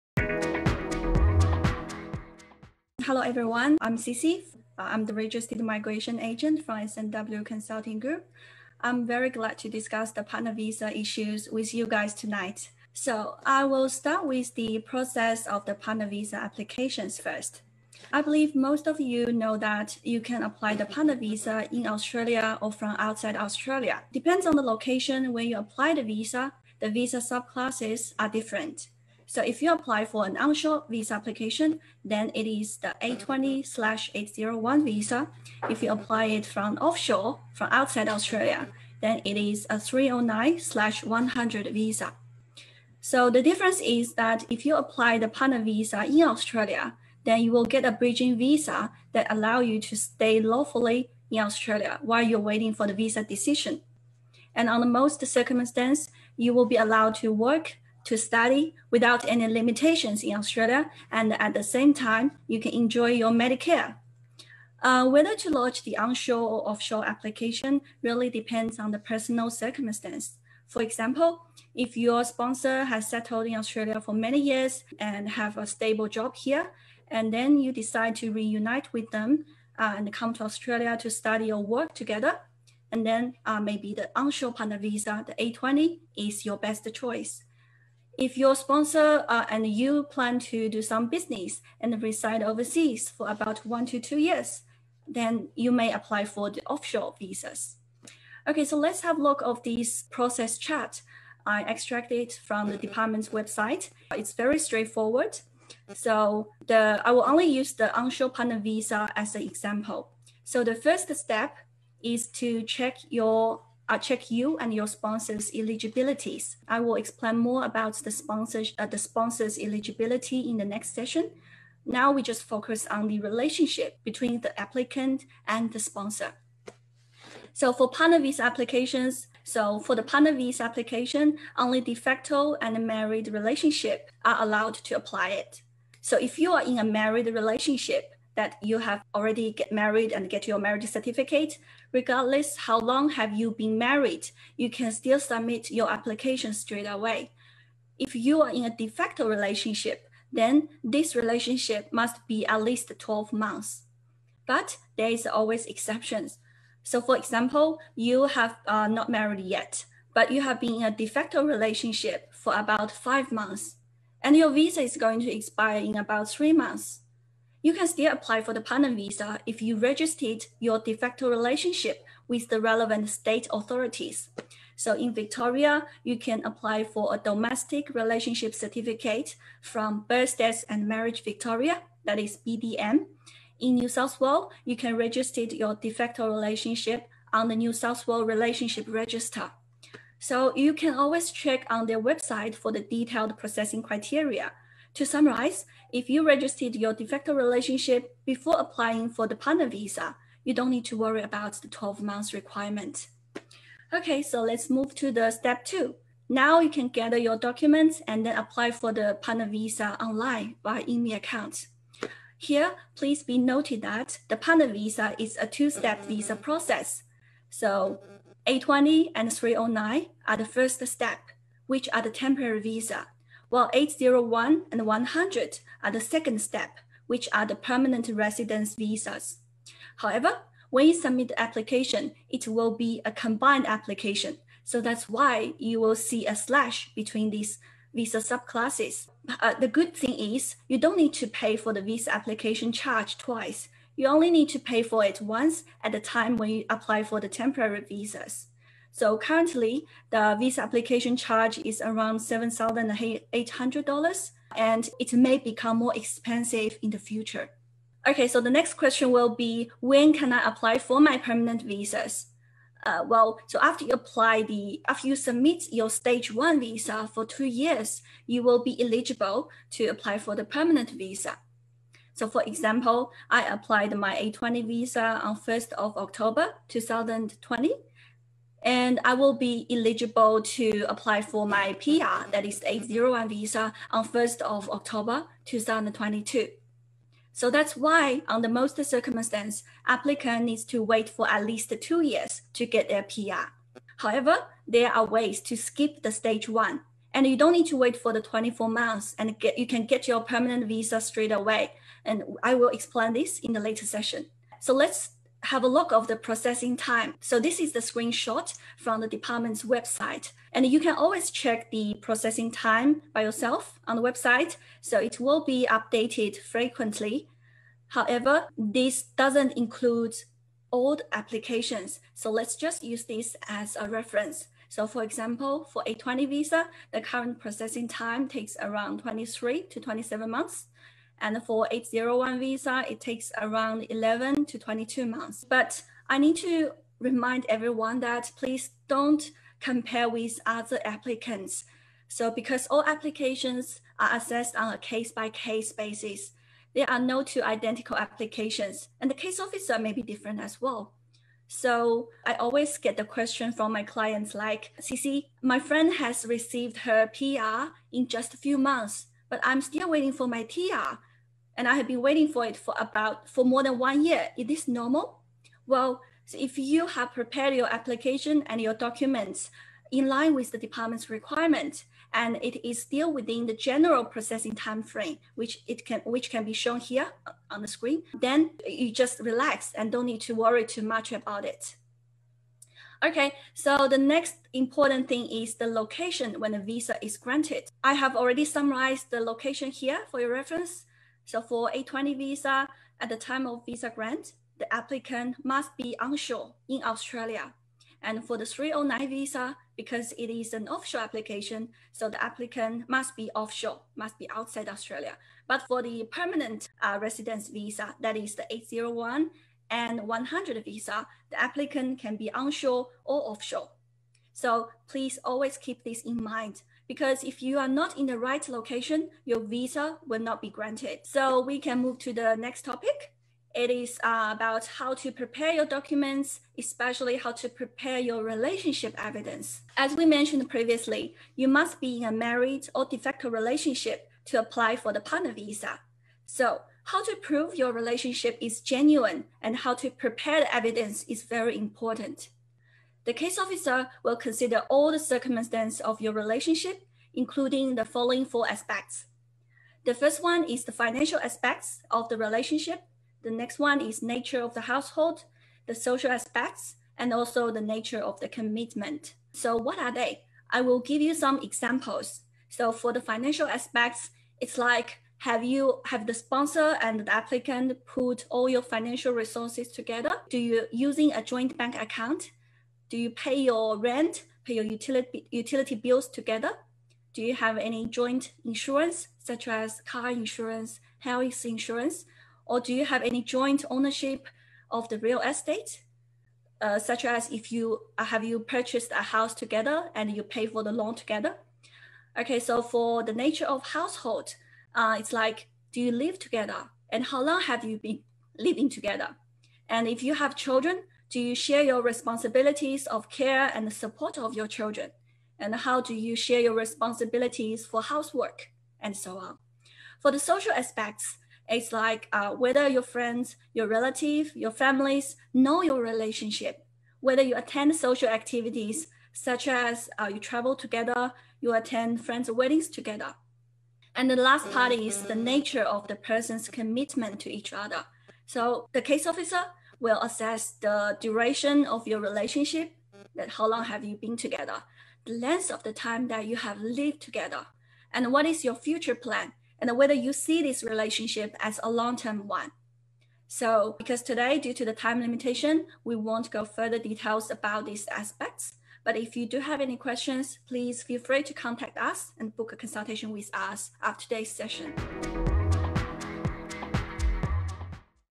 How To Apply For Partner Visa? 820/801 & 309/100
In the Migration program planning levels 2020-21, the migration quota remains unchanged at 160,000. Remarkably, the quota for PARTNER VISA has significantly increased, from 39,799 places in 2019-20 to 72,300 places in 2020-21, accounting for more than 45% of the whole program. Hence, PARTNER VISA is the biggest winner of the Australian Migration Program 2020-21.

In this video, our Registered Migration Agents Cissy will deeply explain the Visa Application and Relationship Evidence for subclass 820/801 and 309/100.

SPEAKER: CISSY WU
- Registered Migration Agent (MARN: 1808880)
- 8 years experience in the Australian Migration industry
- Master of Business, University of Sydney
- Bachelor of Business, RMIT University